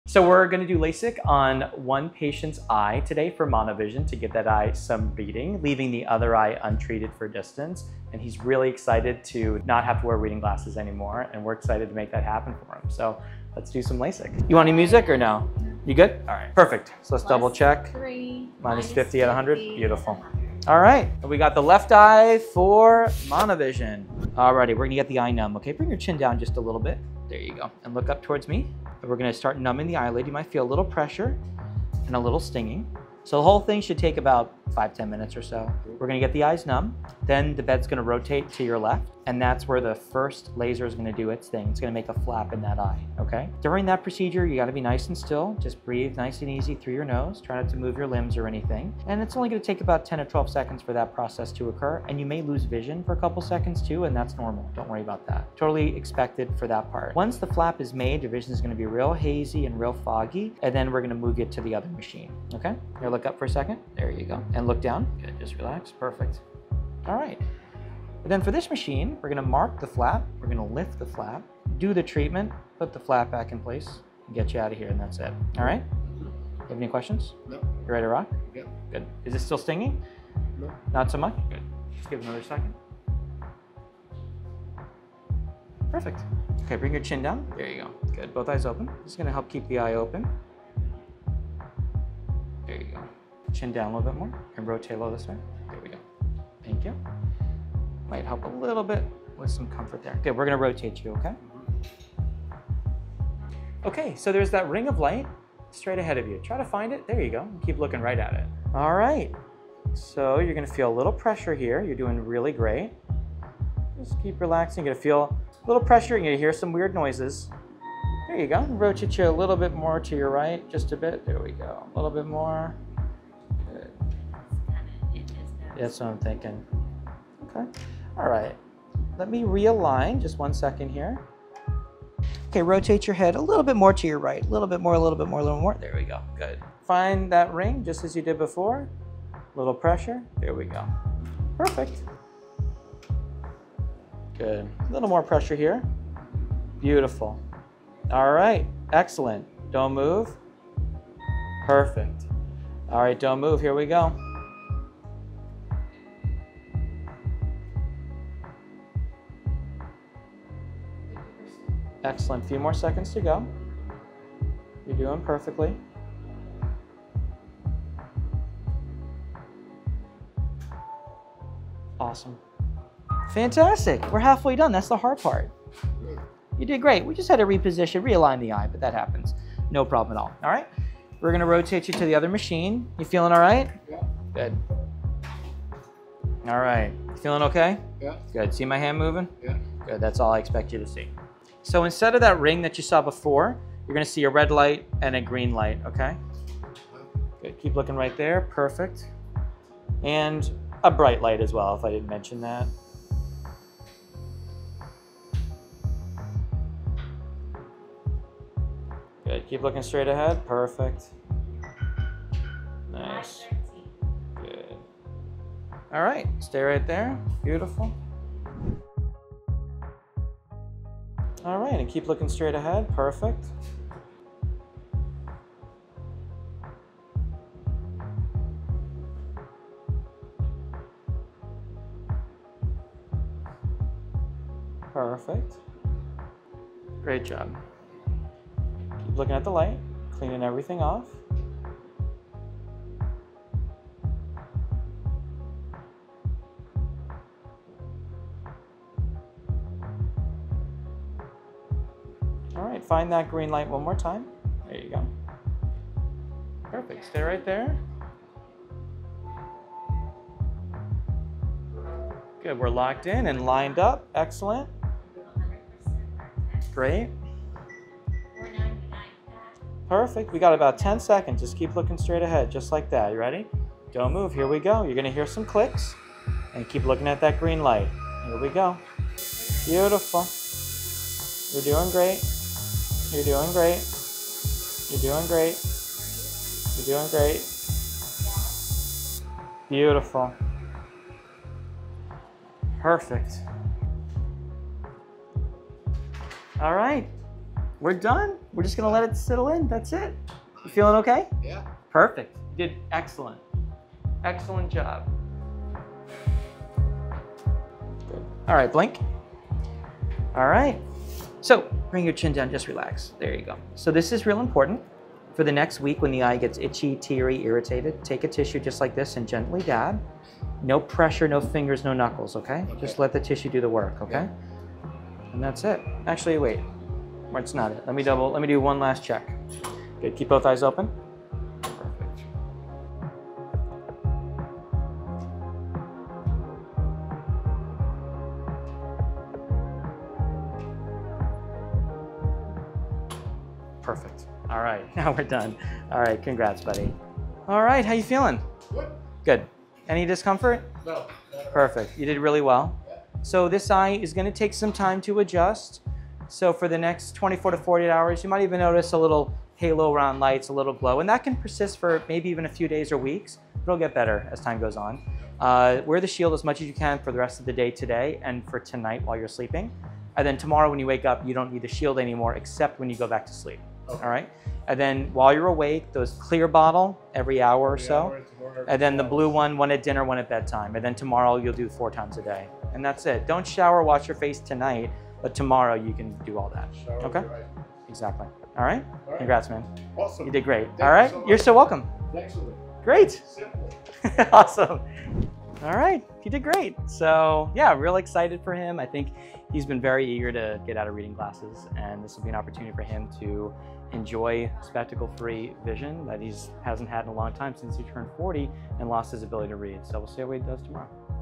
Watch LASIK surgery on ONE EYE improve near vision in minutes!
LASIK using the monovision technique corrects one eye for near vision and one eye for distance. Watch as Dr. Cohen transforms a patient’s vision with this technique at Cohen Laser & Vision Center! ✨